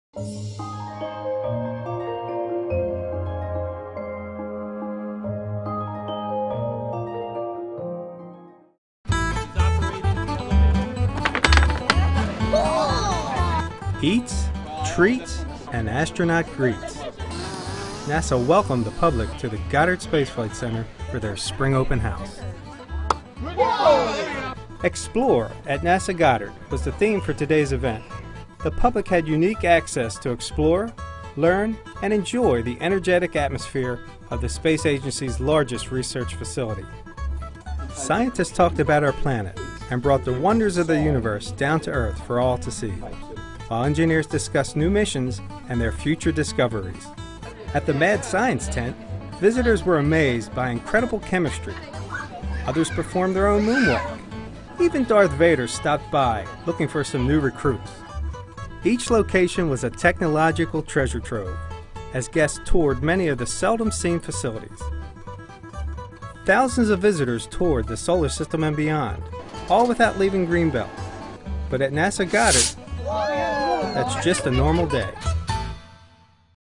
Recap: Explore @NASA Goddard
Thousands visited NASA's Goddard Space Flight Center in Greenbelt, Md., on Saturday, May 14, 2011.